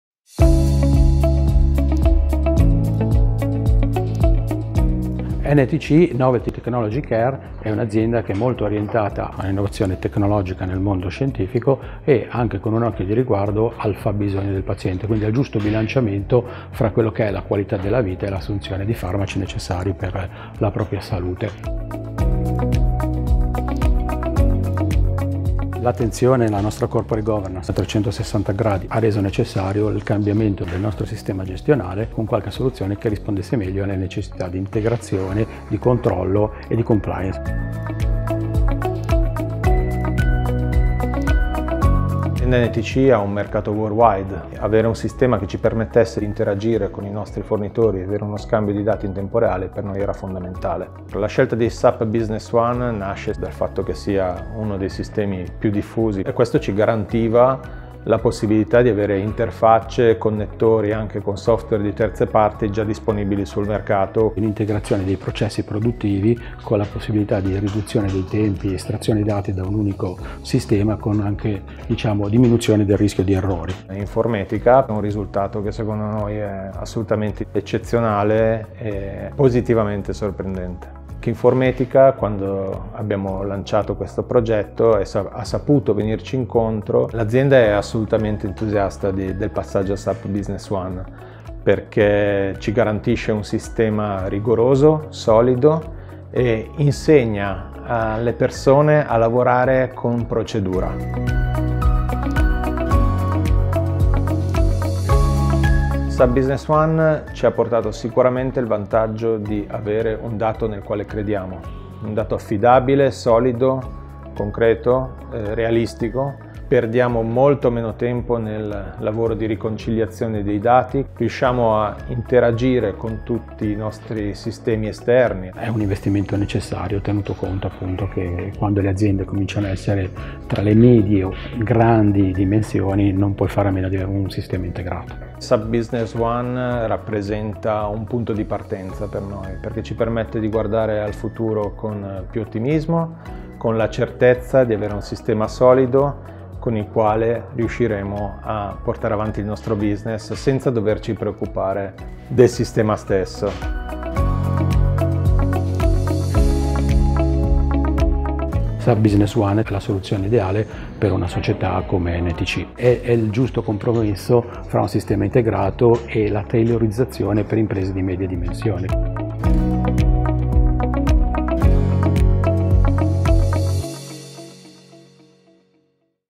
NTC Group, obtains one real-time data certainty with SAP Business One
Find out how NTC, a company that develops and distributes medical devices and medicines with a very sensitivity to the sustainability of materials and packaging, chose to use an international ERP such as SAP Business One to manage the complexity of resources and data through an integrated system.